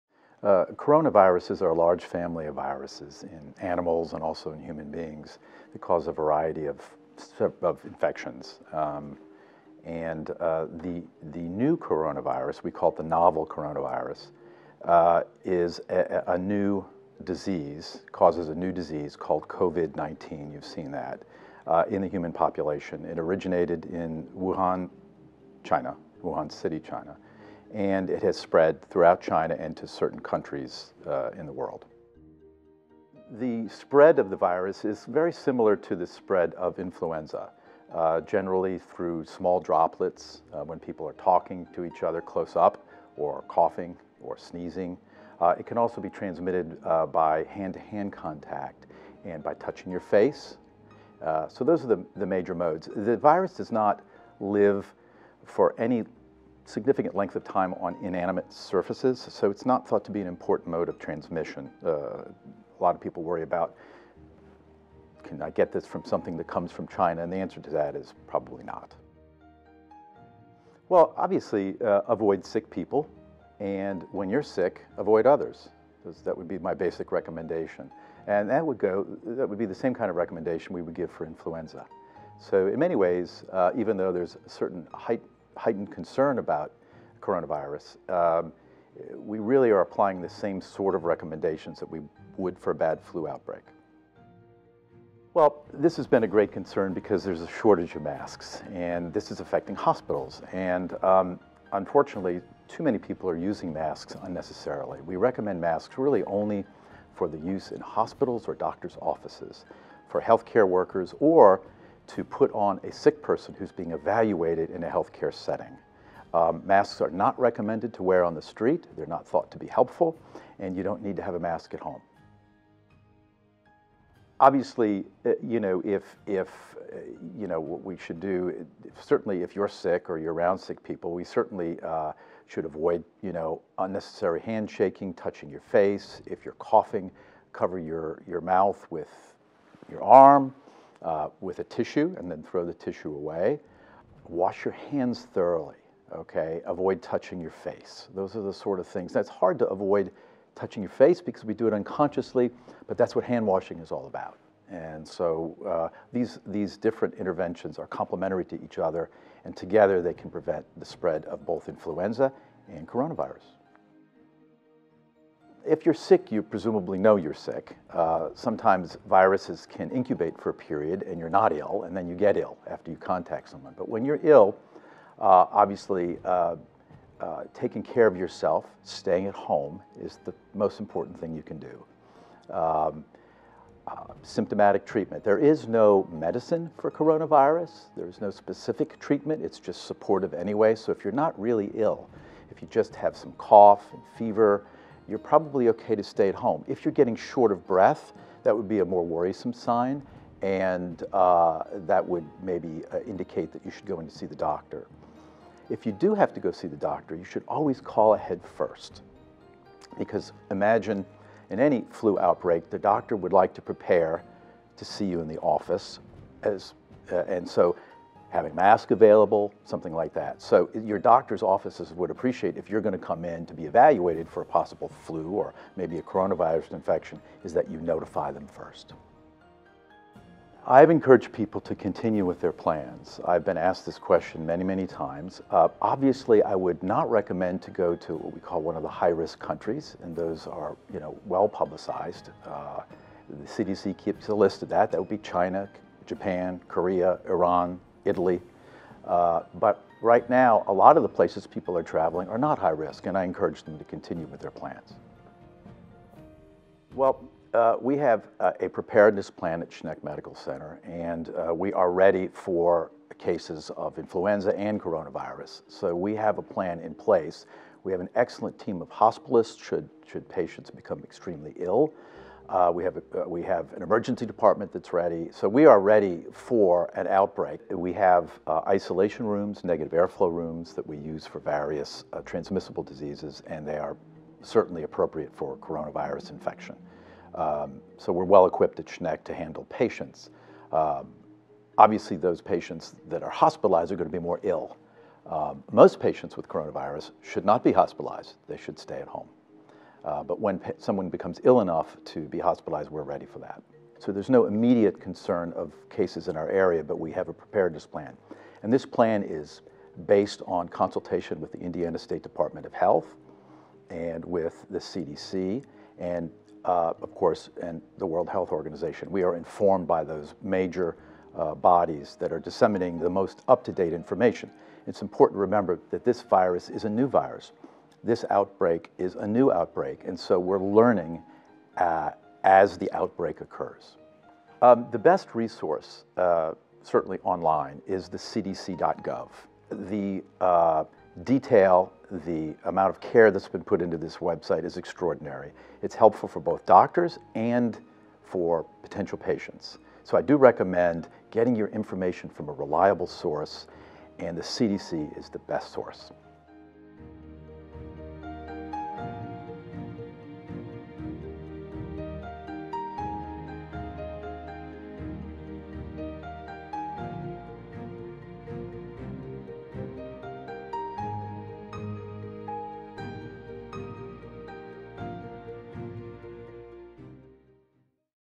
Schneck Medical Center - Get the Facts about Coronavirus
Dr. Christopher Bunce shares facts about the Coronavirus.  Dr. Bunce is an infectious disease specialist with Schneck Medical Center.  He also is the Health Officer for the Jackson County Health Department in Indiana. Learn more about COVID-19 and how you can prevent its spread at SchneckMed.org.